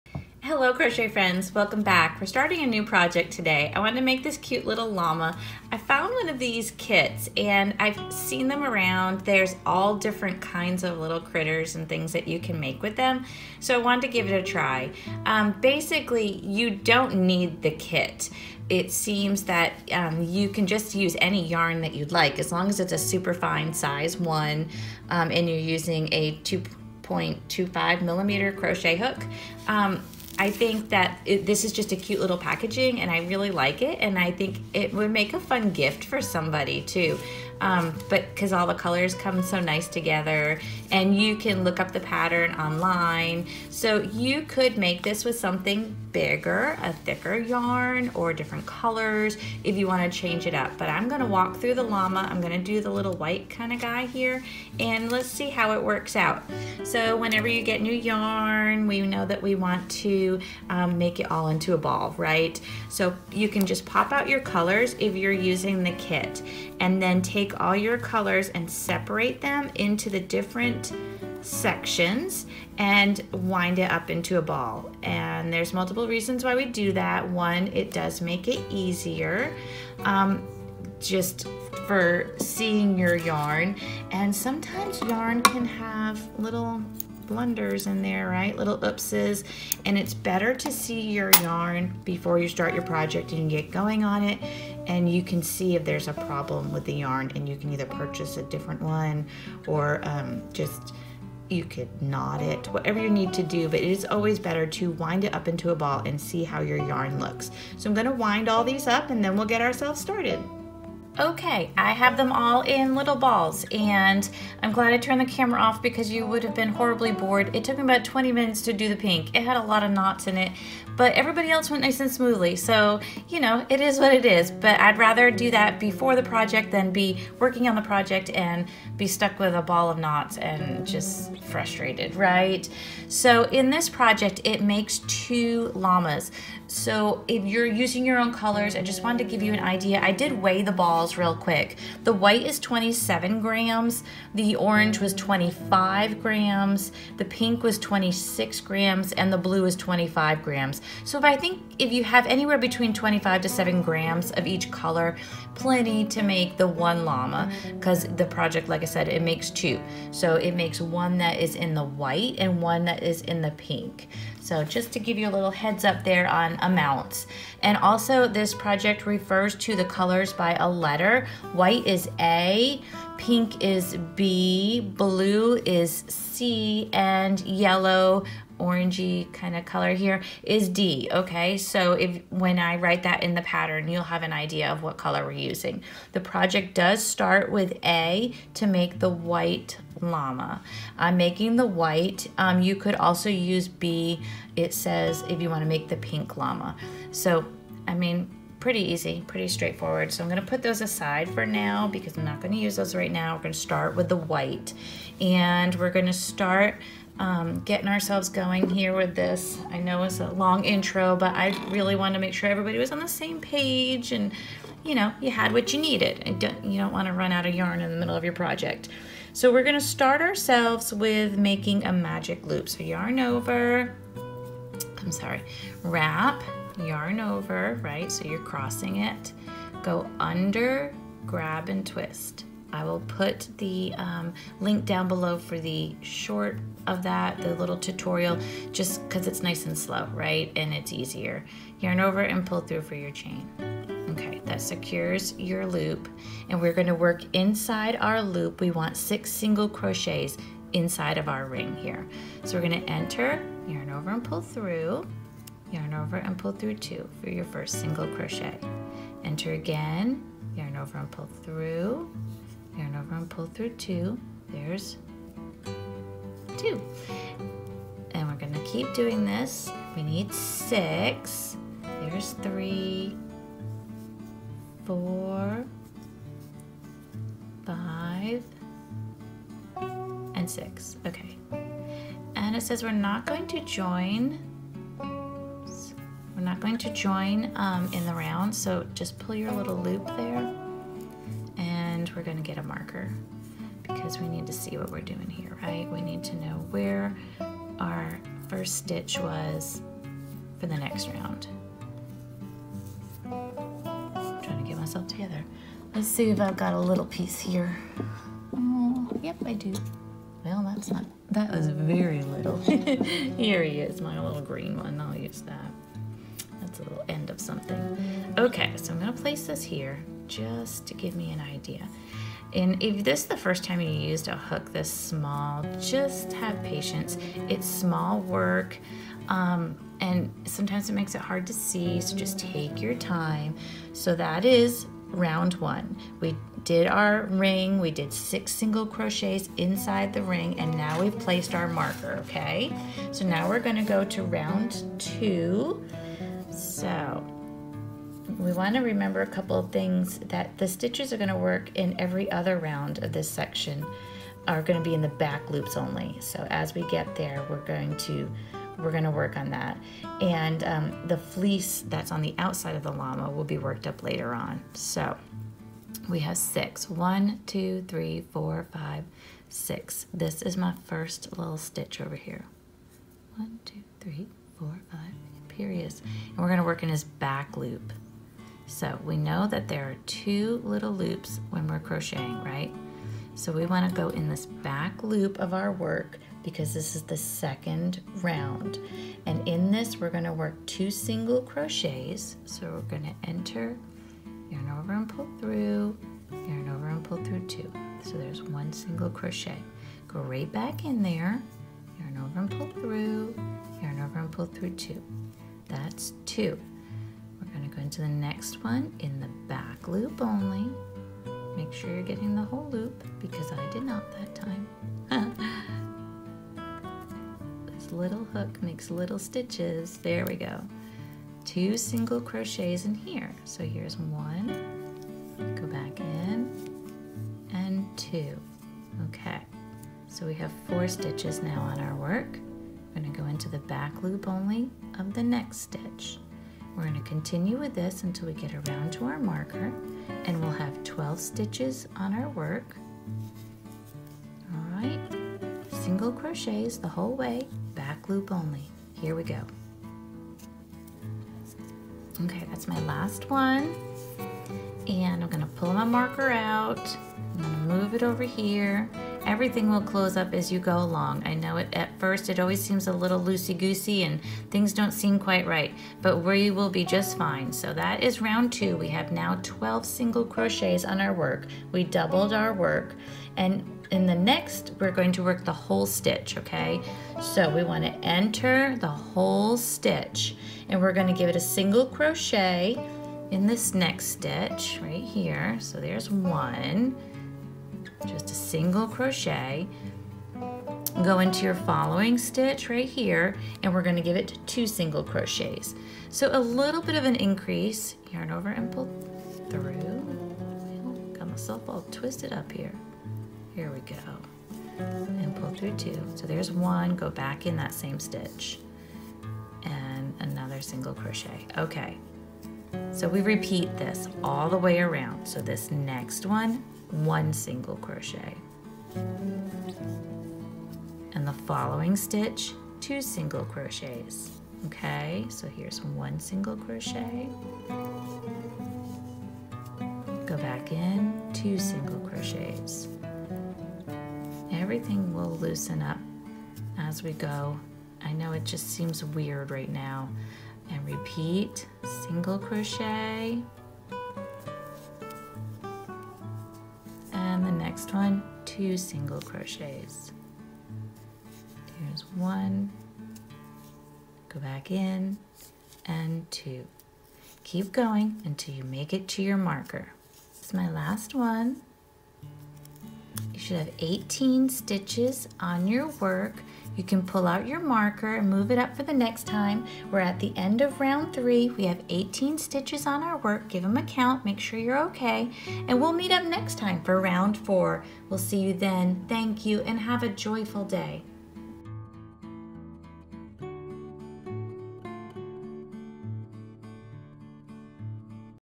Cute Crochet Llama Body Rounds 1-3
You will need:
25-27g white, pink, blue & yellow size 1 yarn
or use Red Heart Larry & Linda Crochet Llama kit
2.25mm crochet hook
yarn needle
embroidery needle
black embroidery floss
6mm safety eyes
drinking straw
poly fil stuffing
scissors
hot glue (optional)

Finished Project Measures Approximately 5” tall x 4” long

Note:
Yarn A= white
Yarn B= pink
Yarn C= blue
Yarn D= yellow

Body:
Using yarn A (white)
Round 1:
magic circle, 6sc in magic loop
do not join, place marker  (6sts)

Round 2:
BLO 2sc in each st (12 sts)

Round 3:
*sc in next st, 2sc in next st rep* around (18sts)